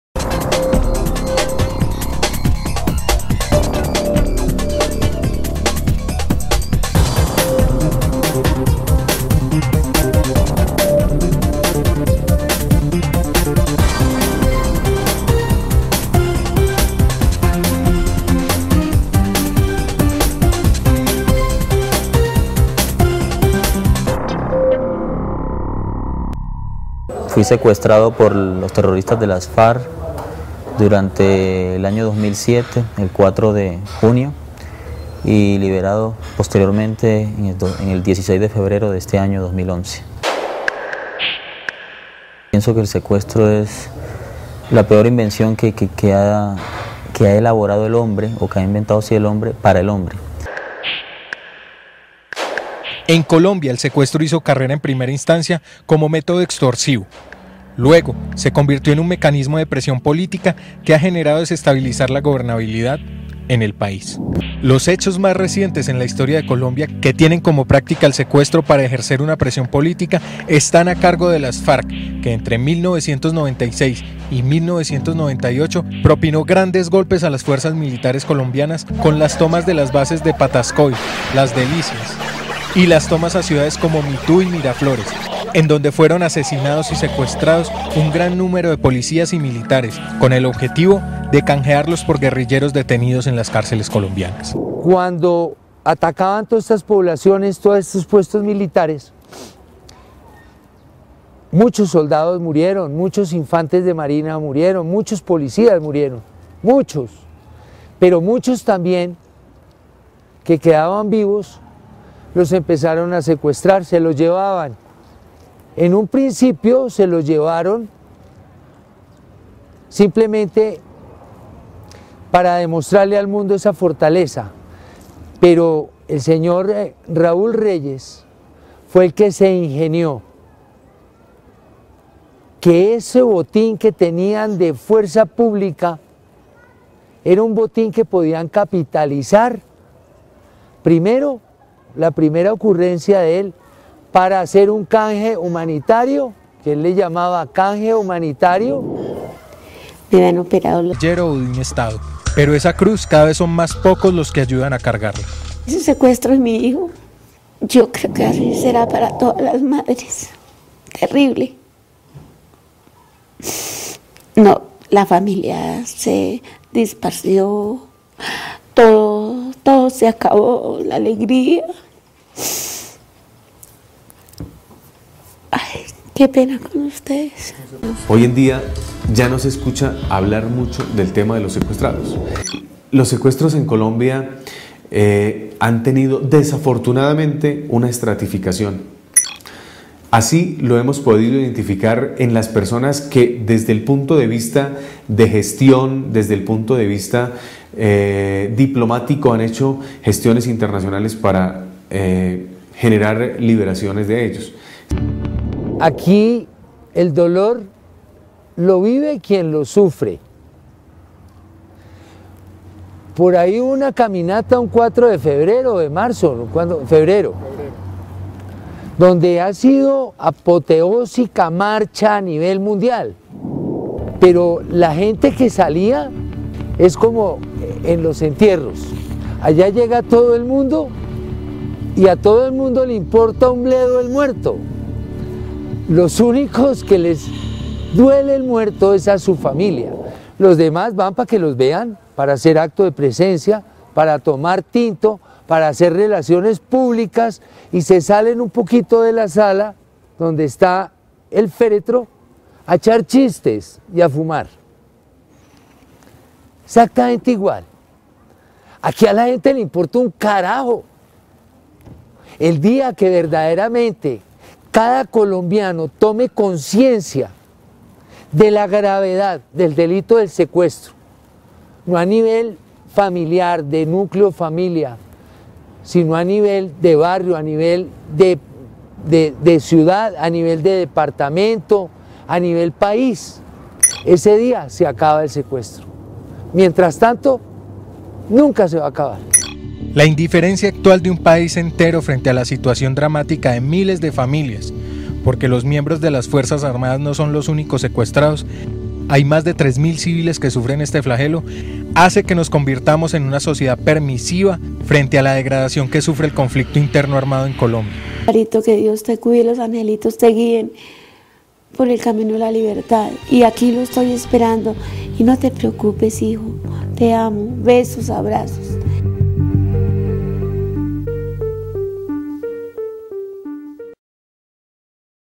PROGRAMA DE TV. CON G. MUÑOZ . EL SECUESTRO ES INDIFERENTE PARA COLOMBIA
GUSTAVO ADOLFO MUÑOZ HABLANDO DE LA INDIFERENCIA SOCIAL CON RESPECTO AL SECUESTRO.